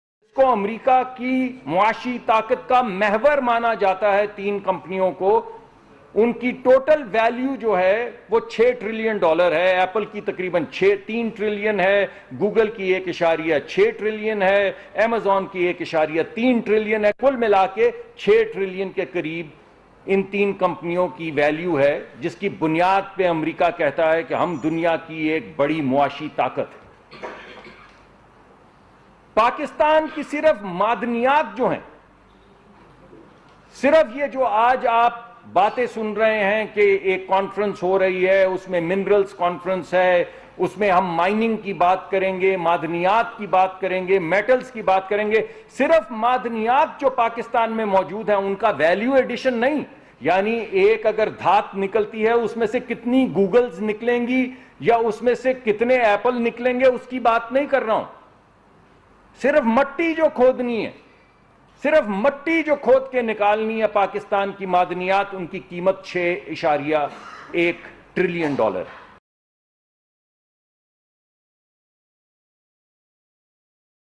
What is the Total value of only the mineral soil of Pakistan?
The total value of the three companies which are considered to be the axis of economic power of the United States is 6 trillion dollars, but the total value of only the mineral soil of Pakistan which is to be mined is 6.1 trillion dollars.